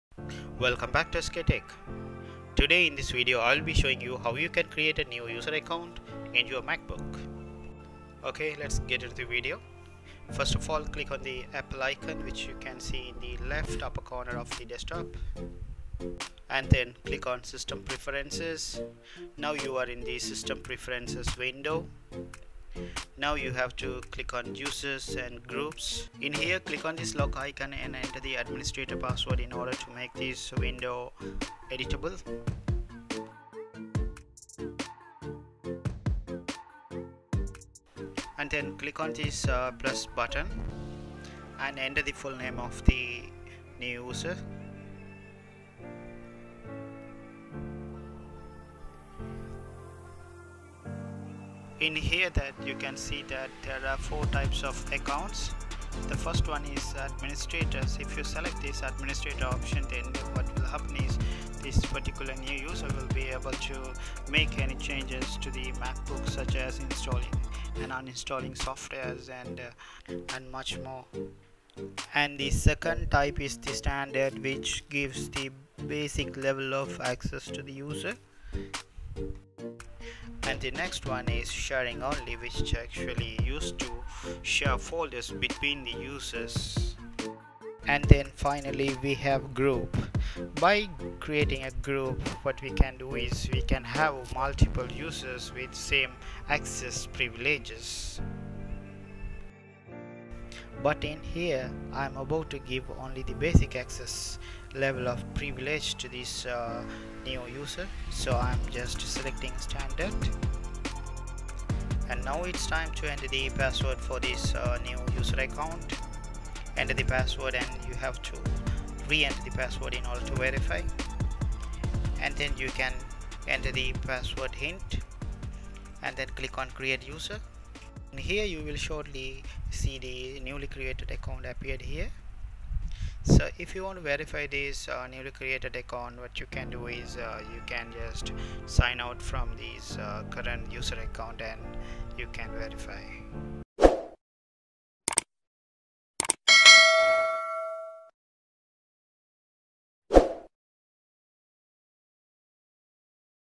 How to Add a New User Account in Macbook Pro | Create Another User in MacBook
In this video i will be showing How to Create Another User in Macbook.

This is an macbook tutrial on how to add another user in macbook just in 2 mins, we hope that after watching this video everyone will be able to Create multiple Users Account in Macbook Pro.
Also we have explained about types of user accounts in macbook and the previlladges.